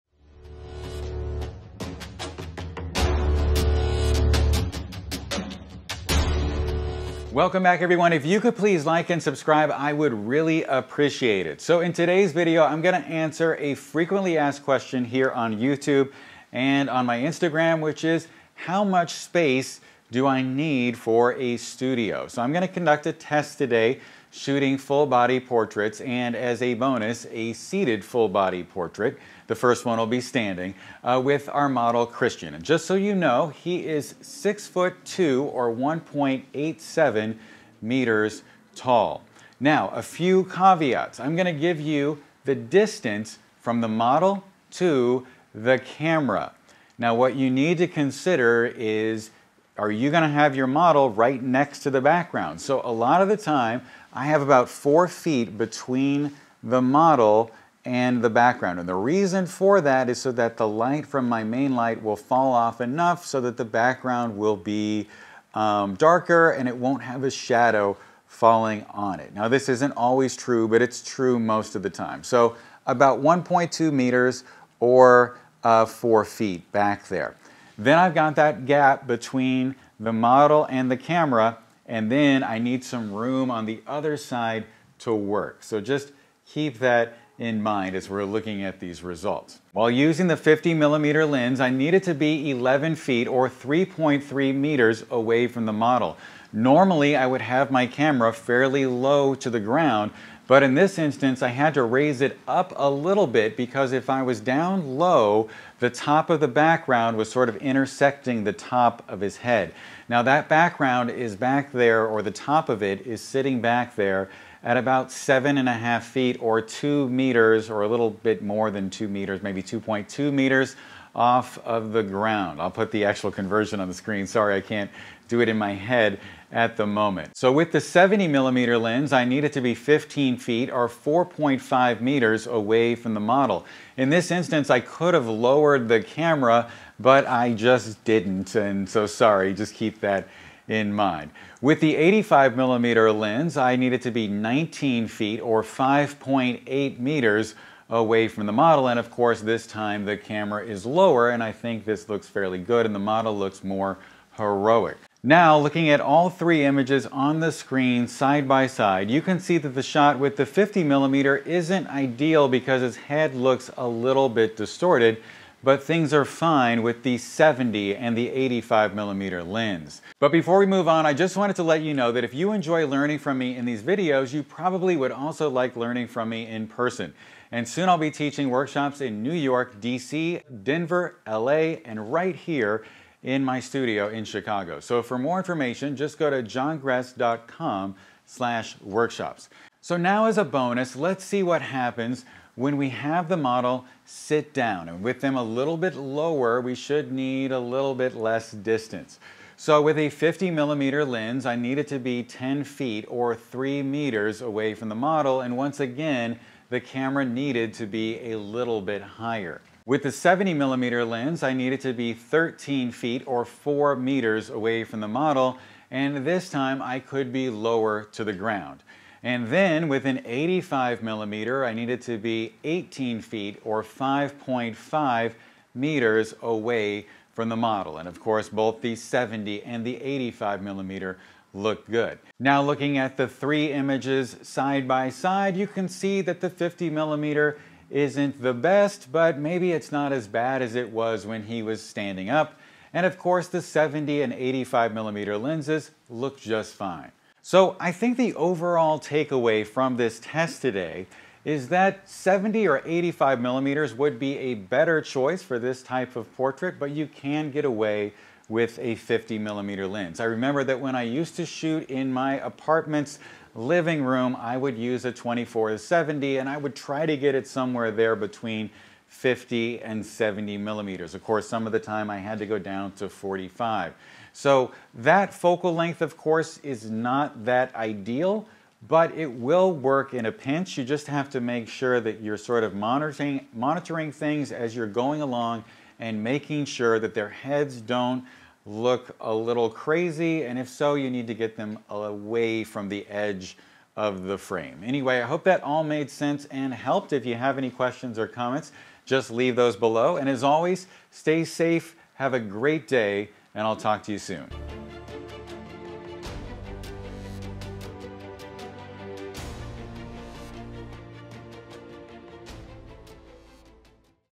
How Much Space Do You Need for Full-Length Portraits? | 50mm vs 70mm vs 85mm Lens Comparison
How much space do you need for full-body portraits in your studio? In this video, I test how much studio depth is required for shooting full-length portraits using 50mm, 70mm, and 85mm lenses. Whether you’re working in a small home studio or a larger professional space, understanding how different focal lengths affect the space needed can help you plan your next shoot.

You’ll also get a bonus test of seated full-body portraits to see how much space you really need when the subject is sitting. This is a must-watch for anyone considering what lens to use for studio photography!

🎥 Gear Used:

• Canon EOS R5
• Canon RF85/1.2 L USM
• RF 28-70mm F2L USM

💡 Lighting:

• Elinchrom ELC 500
• Elinchrom ELC Pro HD 500

Chapters:
00:00 - Introduction
00:11 - Frequently Asked Studio Question
00:24 - Model Details and Caveats
00:47 - Lens Distances: 50mm, 70mm, 85mm
01:12 - 50mm Full-Body Portrait Test
01:42 - 70mm Full-Body Portrait Test
02:13 - 85mm Full-Body Portrait Test
02:45 - Comparing All Focal Lengths
03:00 - Bonus: Seated Portrait Test
03:30 - 50mm Seated Test
03:50 - 70mm Seated Test
04:10 - 85mm Seated Test
04:30 - Comparing Seated Portraits
04:45 - Final Thoughts & Conclusion